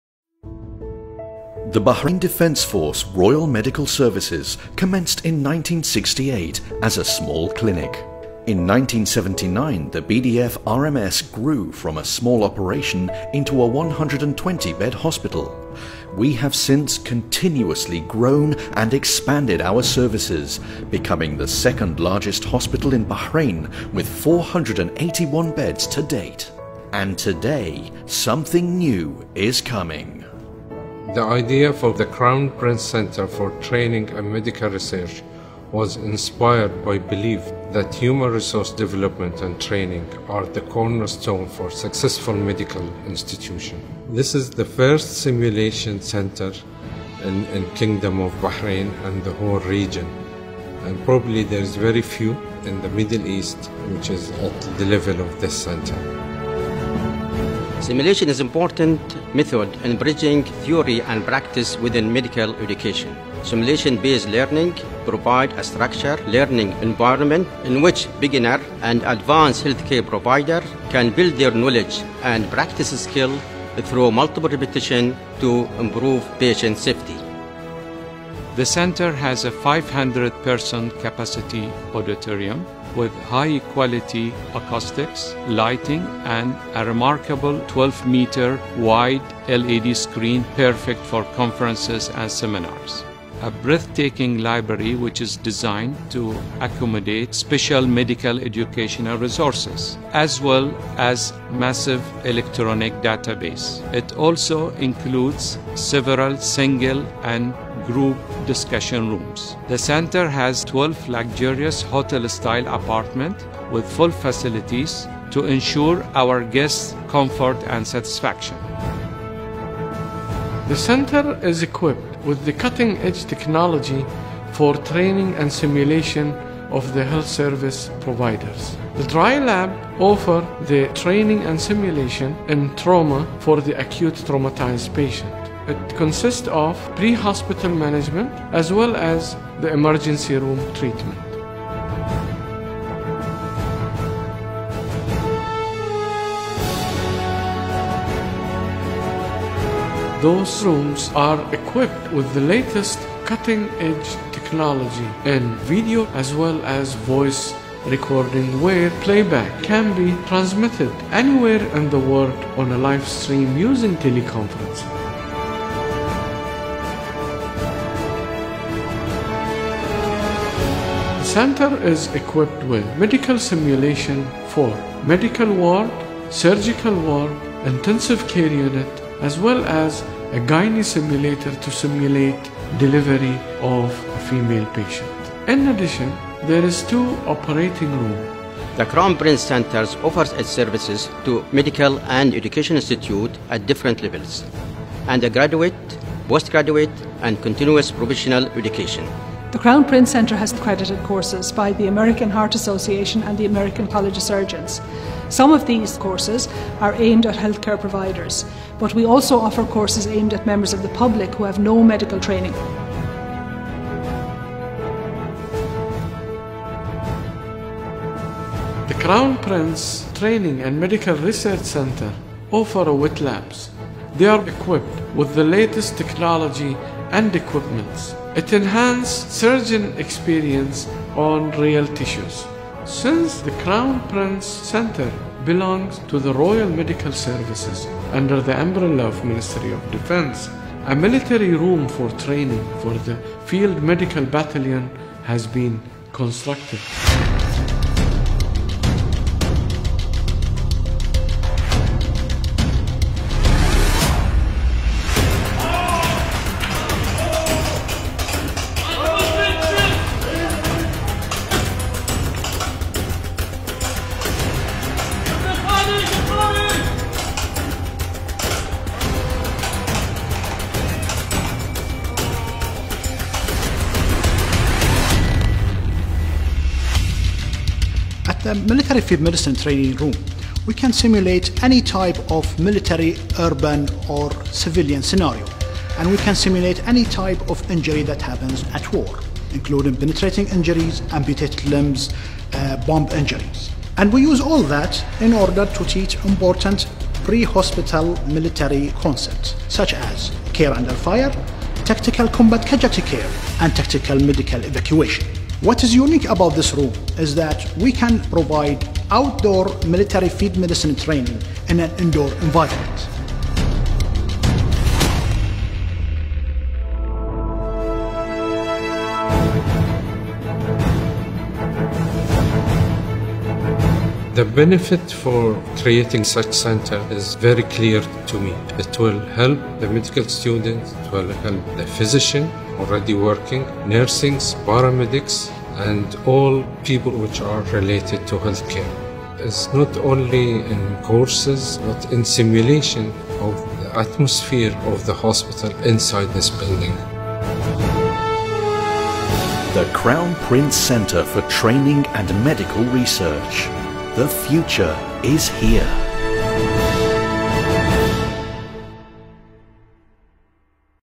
Have you see the amazing Bahrain Defence Force Simulation Centre?
TraumaSim Group were honoured to be invited to visit Bahrain and provided some of our products and training to the newly opened Crown Prince Center for Training and Medical Research, of the BDF Royal Medical Services. This was believed to be the first major dedicated medical training simulation center in the Gulf region at the time